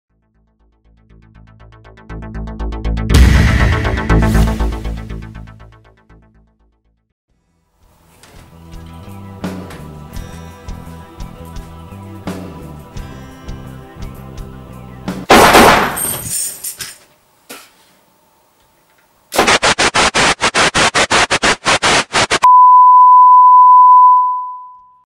After Effects - Dual Battle
Battle between myself.

Gun is KJW G32C blowback airsoft pistol.

All elements with After Effects CS5.

-HellStream Production-